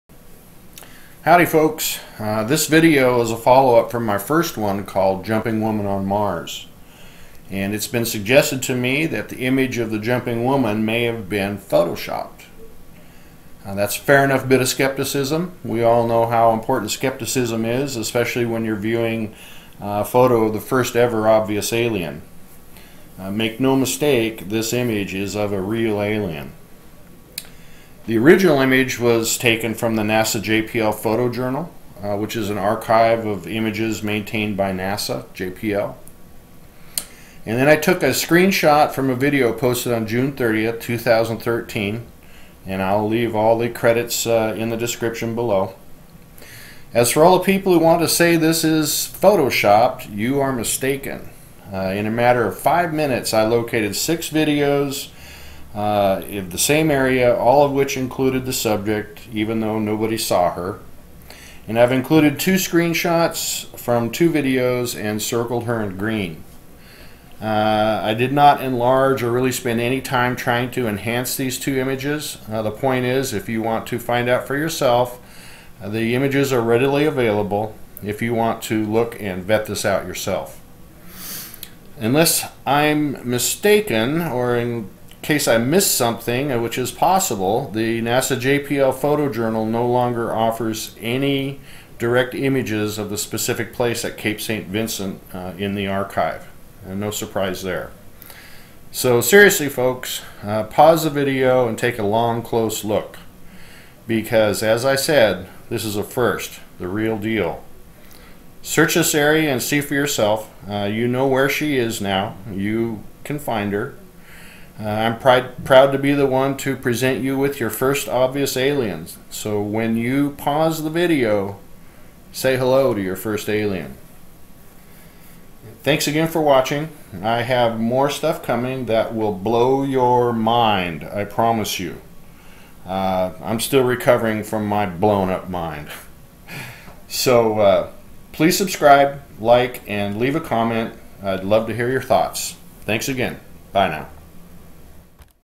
JUMPING WOMAN ON MARS, FOLLOW UP
The original image comes from NASA/JPL, and the subsequent screenshots come from Eagleman 725, June 30, 2013, The Most Important Photo Ever Taken